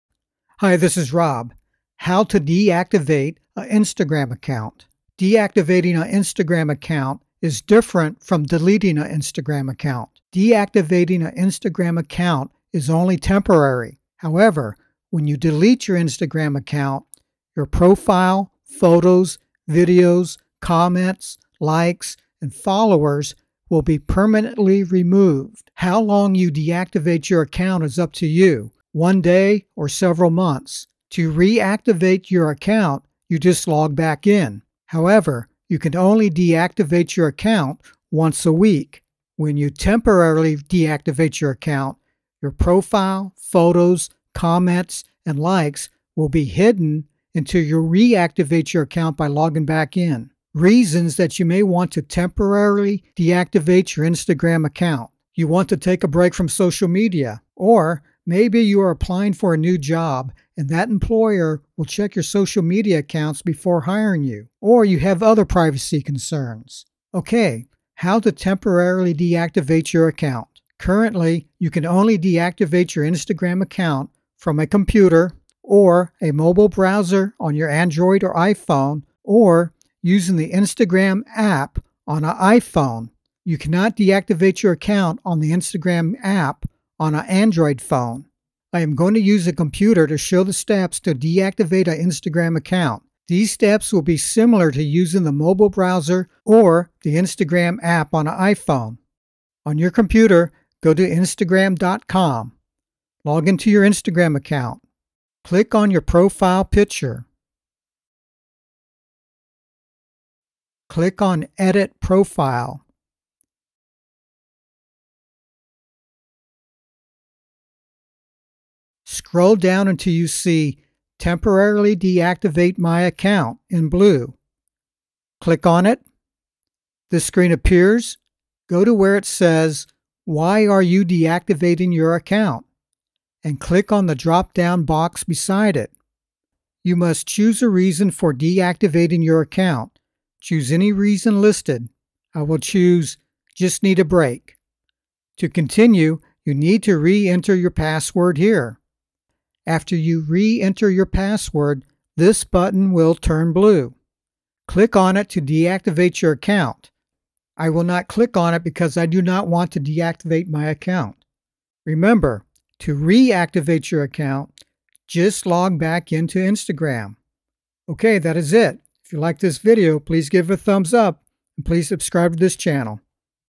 How to Deactivate an Instagram Account
Difference between Deleting and Deactivating. How long Can an Account be deactivated? How to Reactivate an Account. How often can I deactivate my account? What happens during deactivation? Reasons to deactivate? How do I deactivate my Instagram Account?

01:28 Deactivate an Instagram Account using a Computer